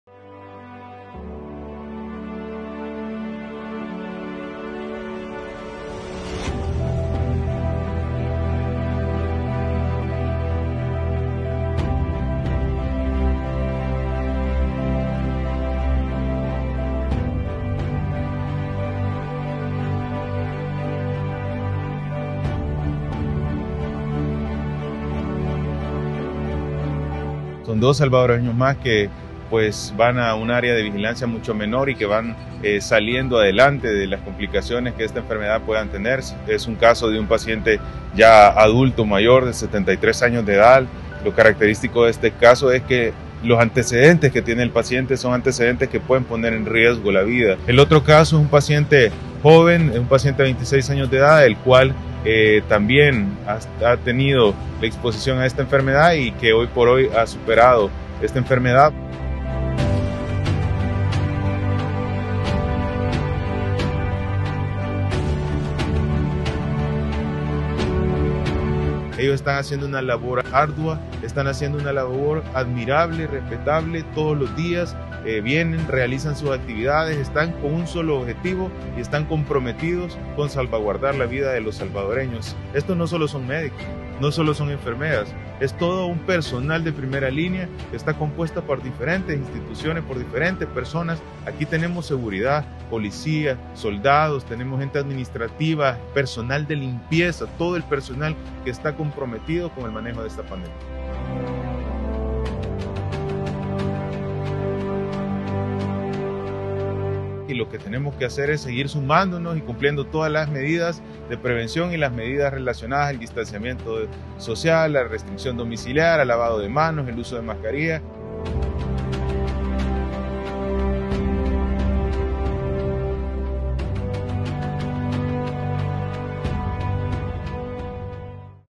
Salud anuncia nuevos pacientes recuperados de COVID-19
AlertaCOVID19SV 📹📺 || El ministro de Salud, Francisco Alabí, anunció que dos pacientes con COVID-19 recibieron el alta, luego de superar la enfermedad, en el hospital San Rafael de Santa Tecla. Se trata de dos personas, una de 26 años y otra de 73. Ambas estuvieron en cuidados intensivos.